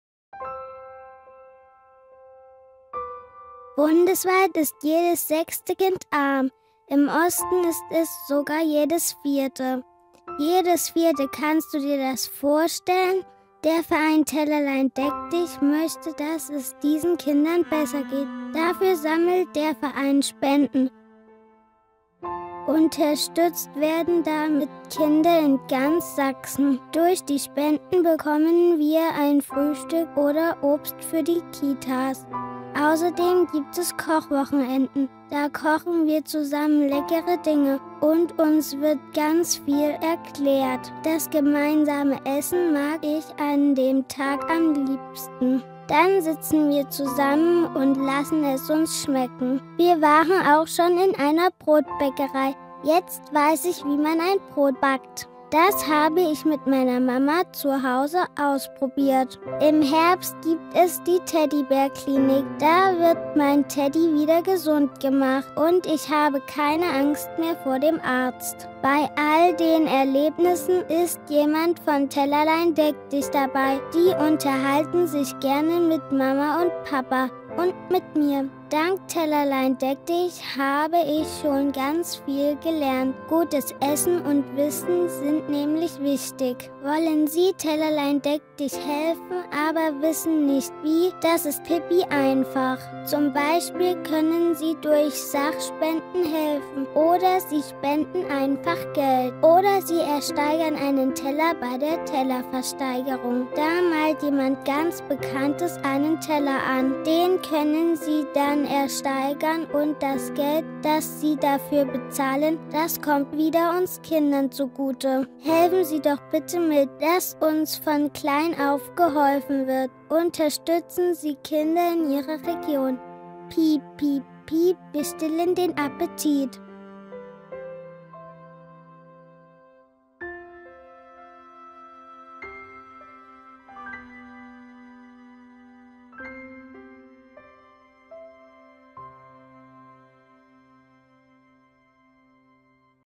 Imagefilm - Tellerlein deck dich ► Crossmedia 2016
Tellerlein deck dich hilft bedürftigen Kindern in Sachsen. Mit Spenden organisiert der Verein ein Frühstück, Obst für den Kindergarten oder die Teddybärklinik.

Mehr Informationen zum Crossmedia-Modul für Medienstudenten an der Hochschule Mittweida sowie den aktuellen Kampagnen: